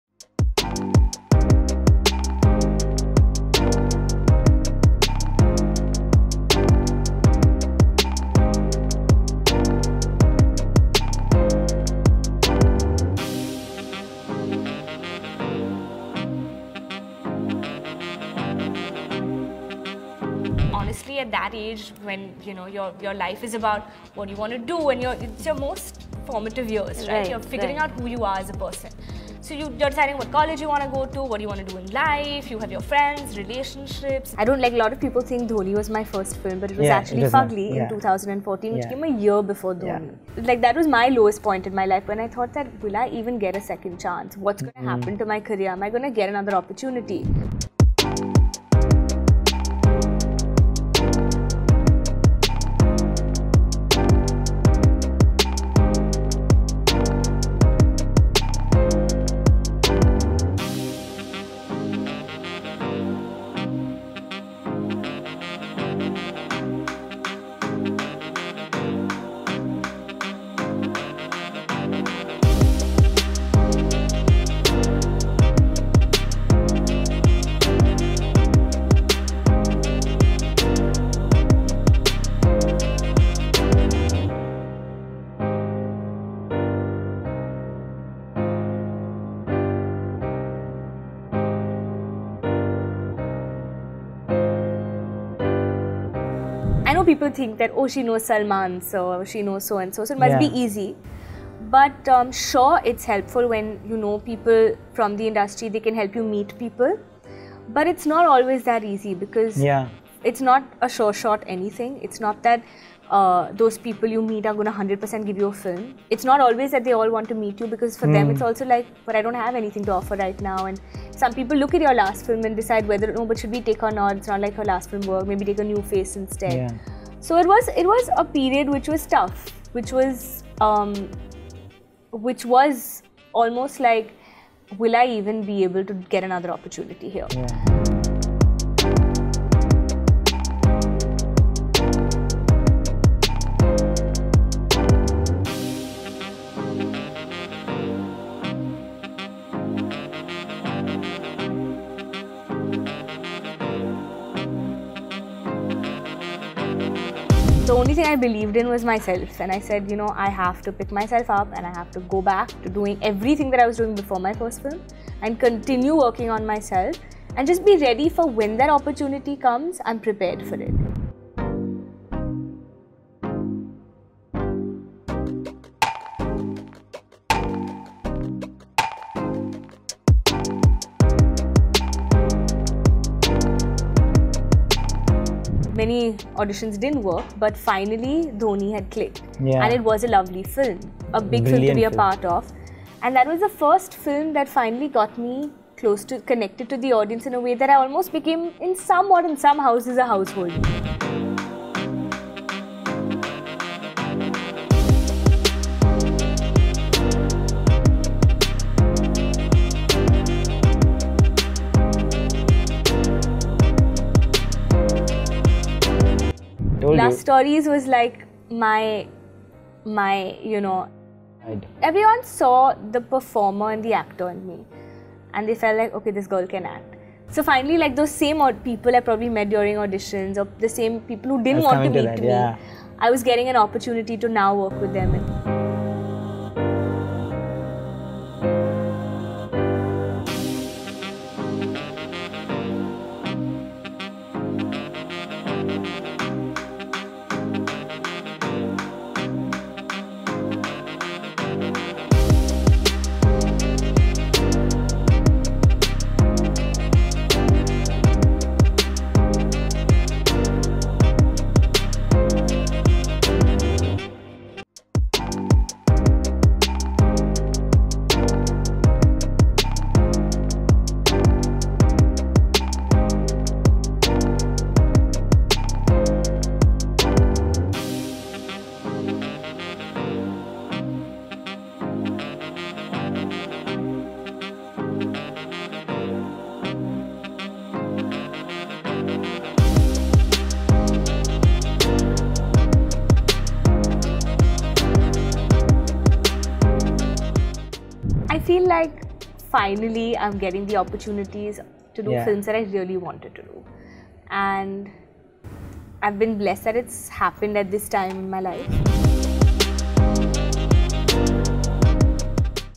Kiara Advani talks about her struggles post her debut in Bollywood; WATCH this video to know more
Kiara Advani is soaring high with success. Kiara has won over Bollywood fans with her recent performances. But things have not always been a cake walk for the actress. Watch this video where Kiara Advani reveals about all the struggles she faced on the way to reach where she is today.